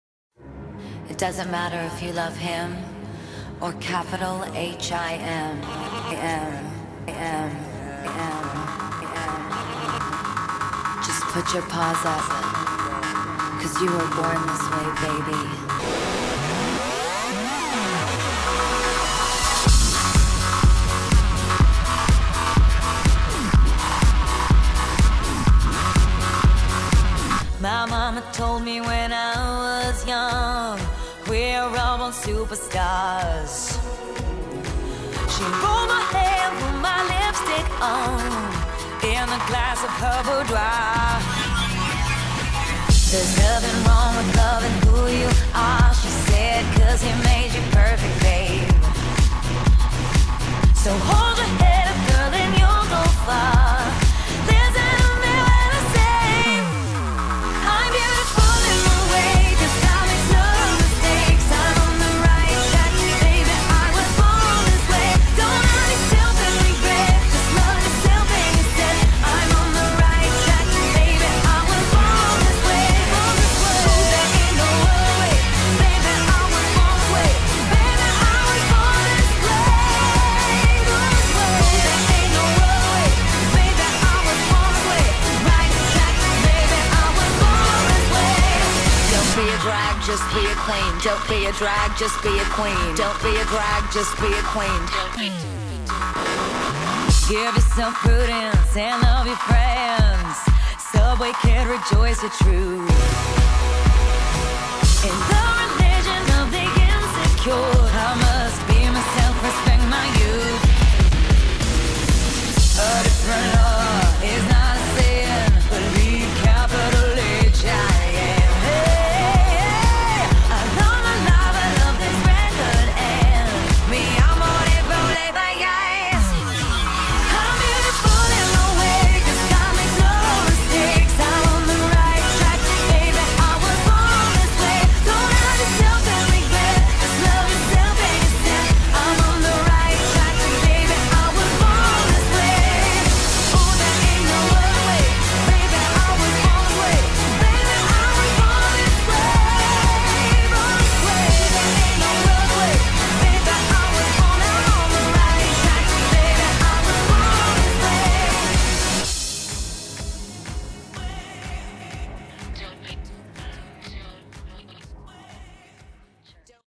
her space speedpaint
Materials/Tools used: pencil and Dr. Ph. Martin's watercolour on bleed proof paper. she turned out better then I thought.